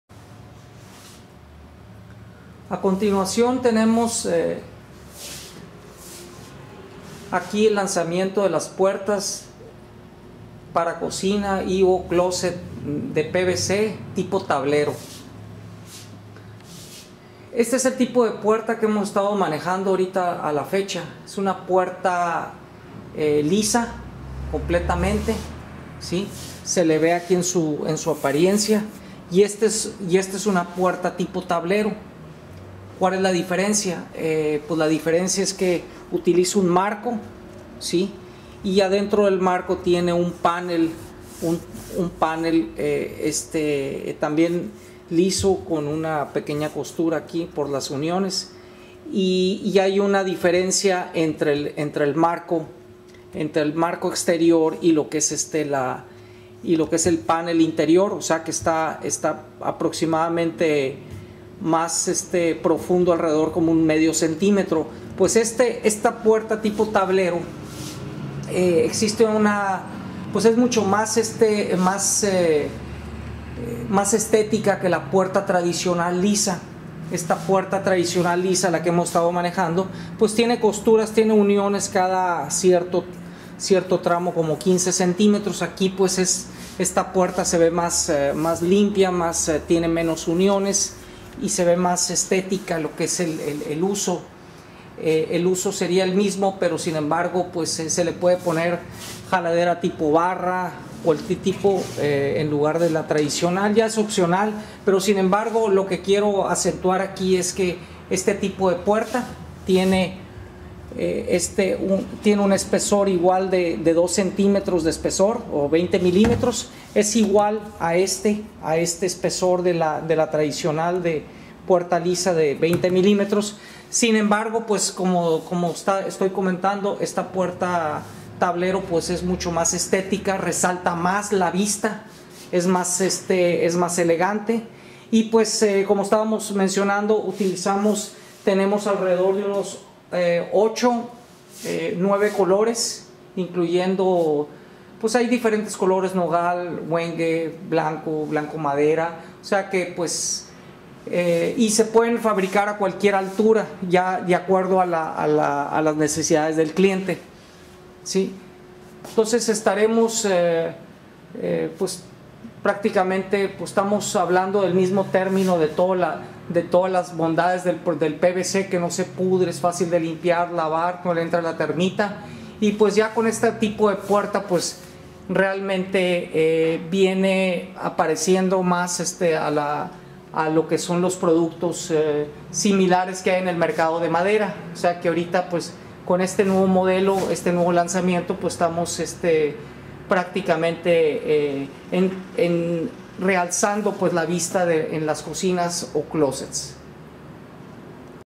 PUERTAS DE PVC PARA MUROBLOCK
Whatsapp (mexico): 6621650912
Las Puertas de PVC para MuroBlock las fabricamos a la Medida de sus necesidades.
Hacemos una plantilla de su cocina, y fabricamos marcos de 20 centimetros de espesor (El cual le da una resistencia muy superior a materiales mas delgados que se utilizan actualmente en el mercado).
En la puerta utilizamos un espesor de 20 milimetros si se trata de una Puerta Lisa, y Si el cliente desea una puerta mas estetica, podemos usar la nueva Puerta Tipo Tablero que le da mas elegancia a su cocina o closet.
Contamos con amplia variedad de colores disponibles.
Son muy durables, resistentes y no les afecta la termita, moho, humedad o salitre.
Ideales para todo tipo de clima.